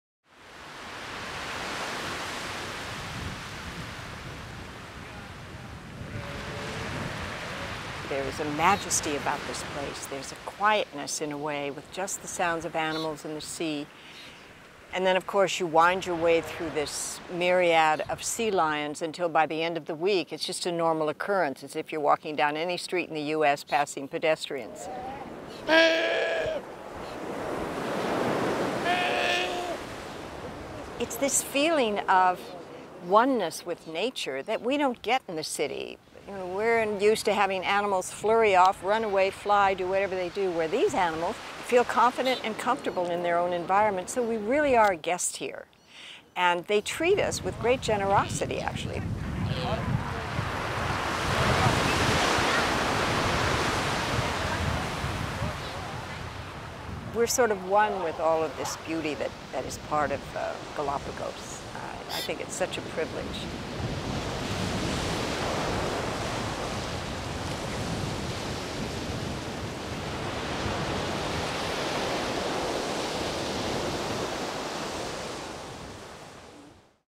Jan Cousteau on Exploring the Galápagos Islands
National Geographic Expert Jan Cousteau reflects on exploring the Galápagos Islands with Lindblad Expeditions-National Geographic.